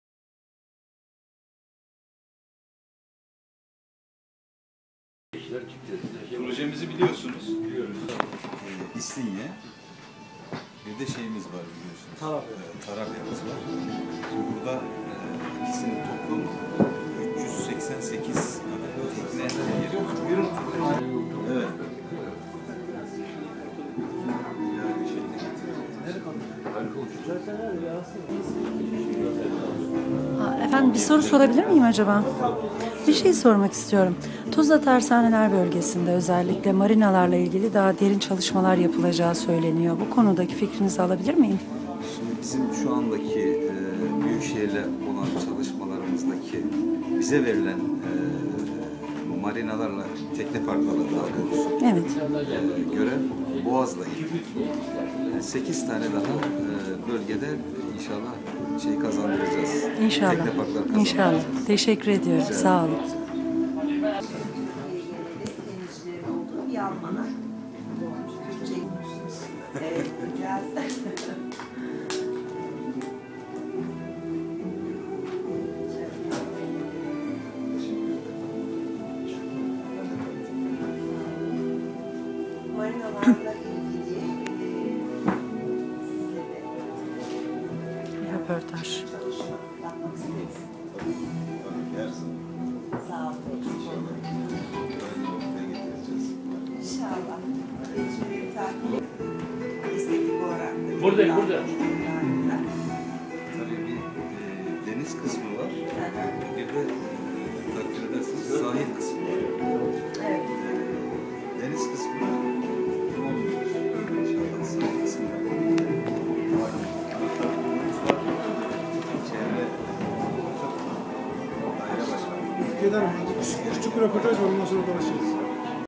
İSPARK GENEL MÜDÜRÜ MEHMET ÇEVİK VE TEKNE PARK PROJESİ
İspark Genel Müdürü  Mehmet Çevik elyacep.tv'ye konuştu.